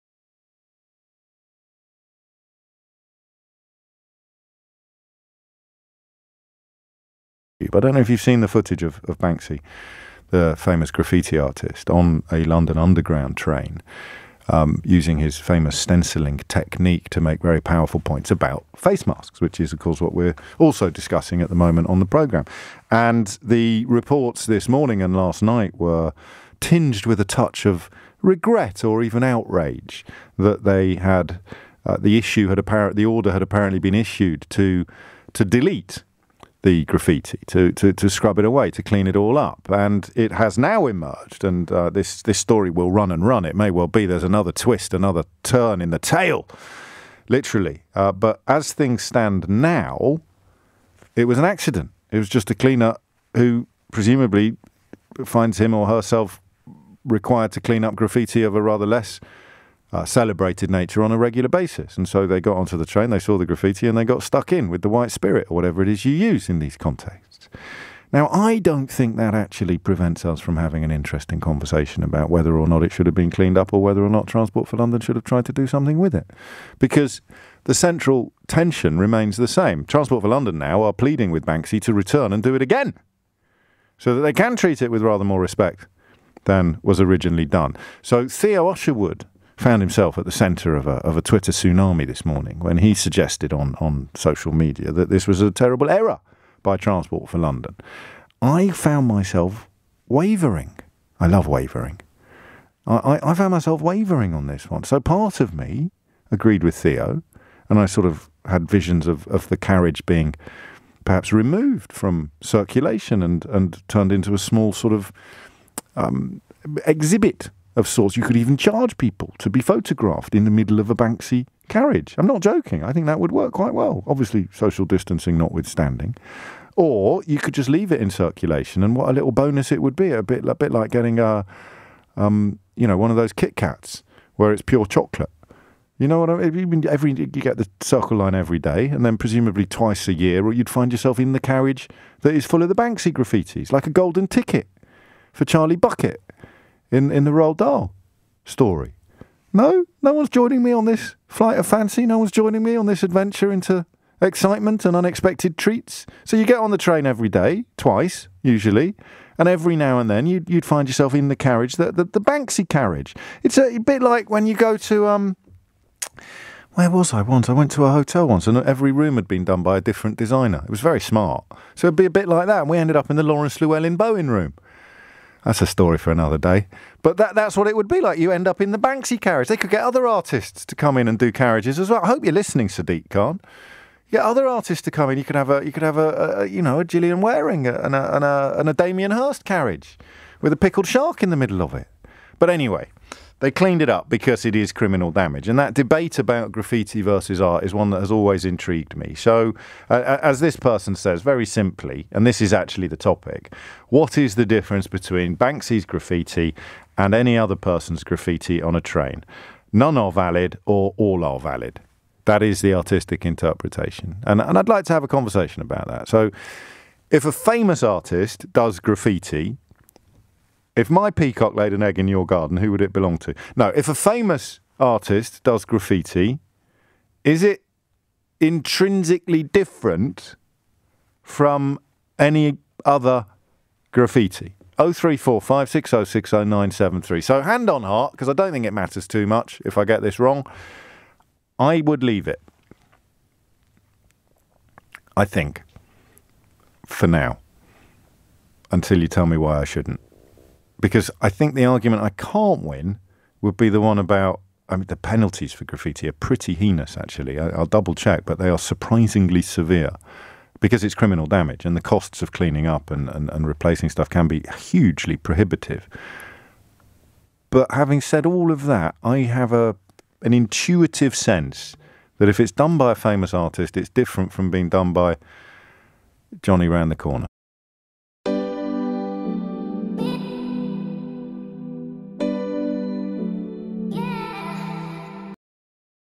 James O'Brien on why Banksy's graffiti should have remained on tube carriage
James O'Brien on why Banksy's graffiti should have remained on tube carriage. After much thought, James O'Brien explained why he thought Banksy's art should have remained on the tube carriage.  In his familiar political style, Banksy crafted a rat sneezing green droplets across the train window, indicating the spread of 'LURGI'. As always Banksy left no trace of his identity other than his signature Style artwork. Transport for London has said the art was removed due to the anti-graffiti policy but welcomed Banksy. Banksy covered the inside of a London Underground carriage with coronavirus inspired art but it was cleaned off by TfL for breaching its anti-graffiti policy. The reputed street artist uploaded a video online with the caption, ‘If you don’t mask – you don’t get', which showed him spraying famous rats inside a London Underground Tube carriage on the Circle Line.